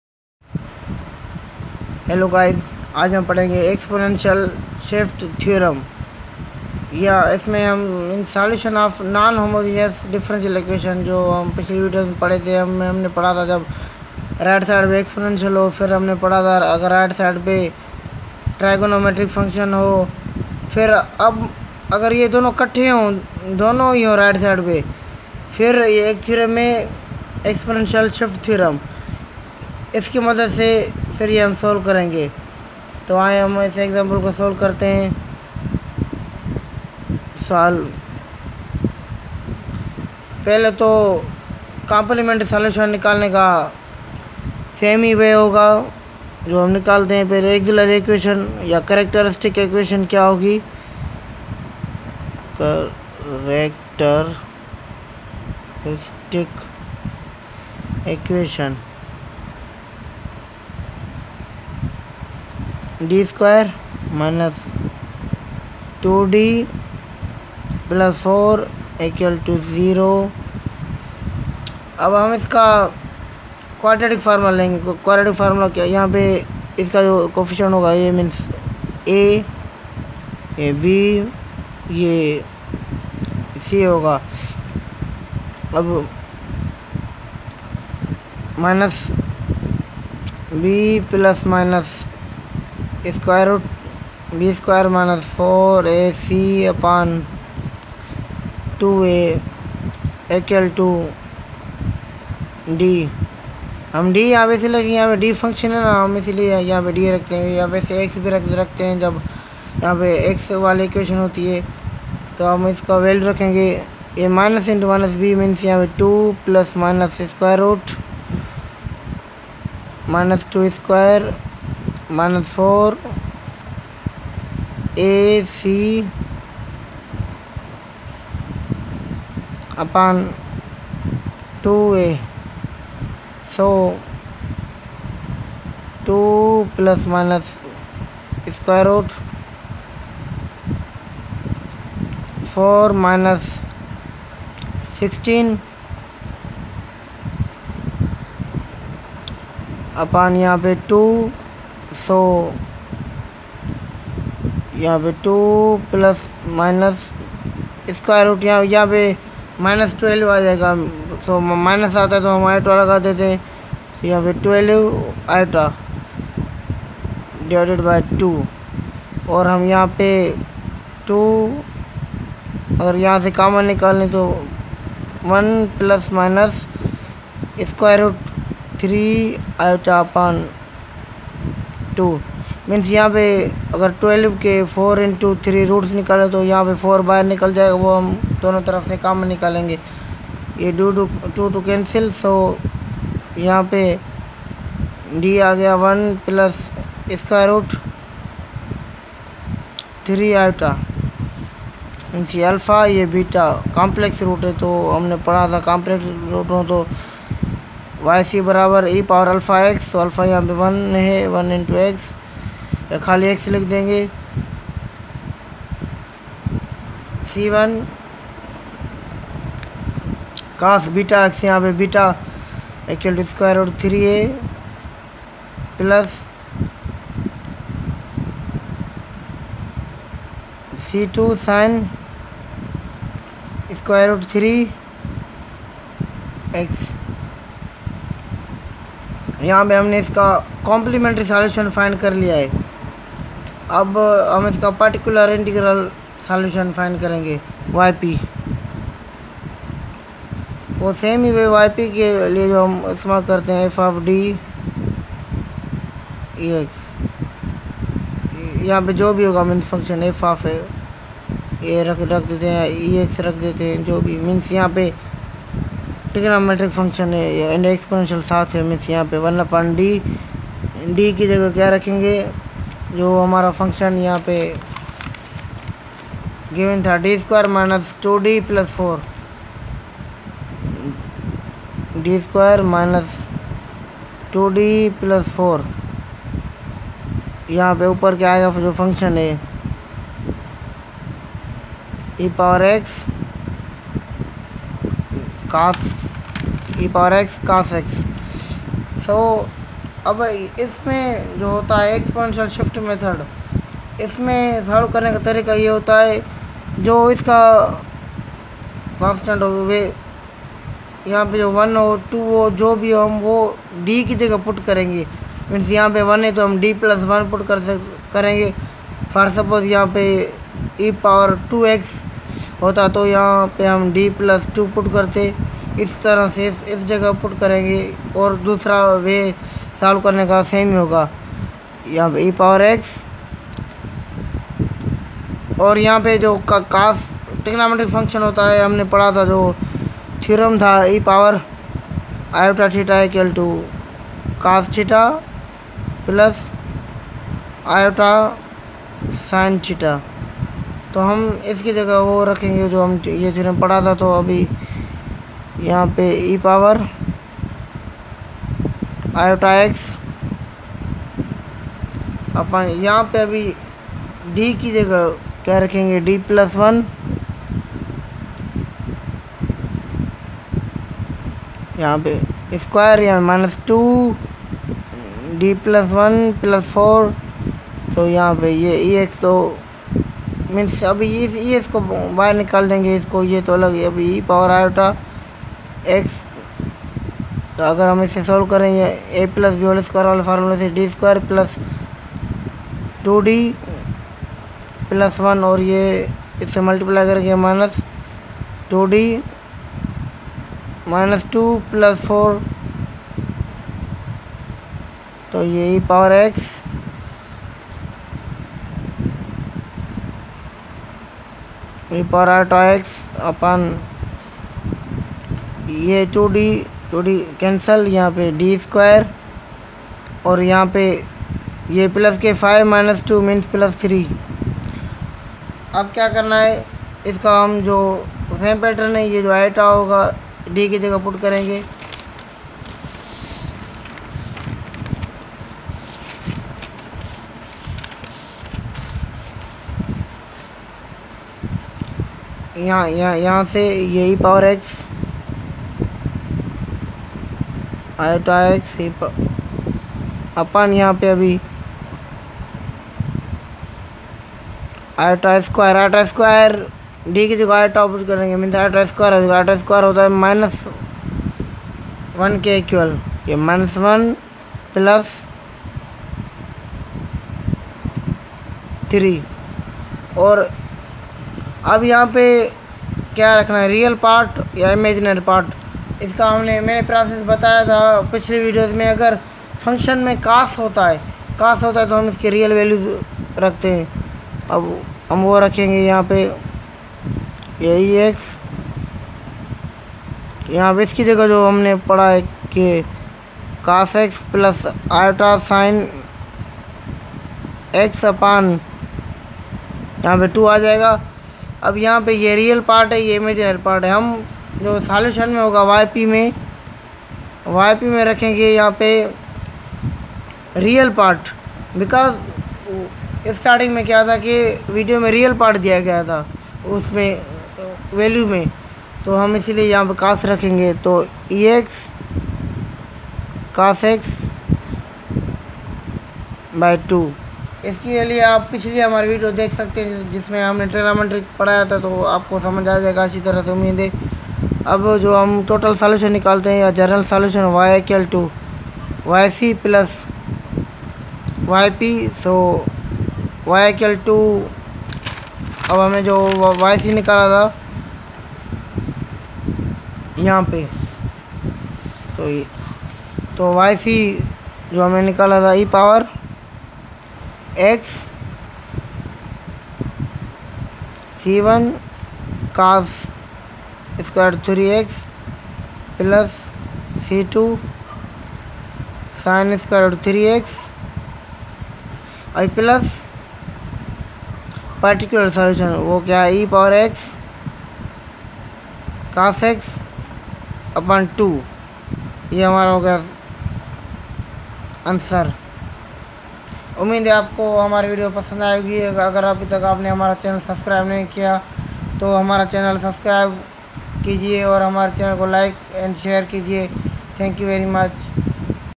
Differential equations: exponential shift thereon
Solution of non homogenous linear differential equations by exponential shift thereon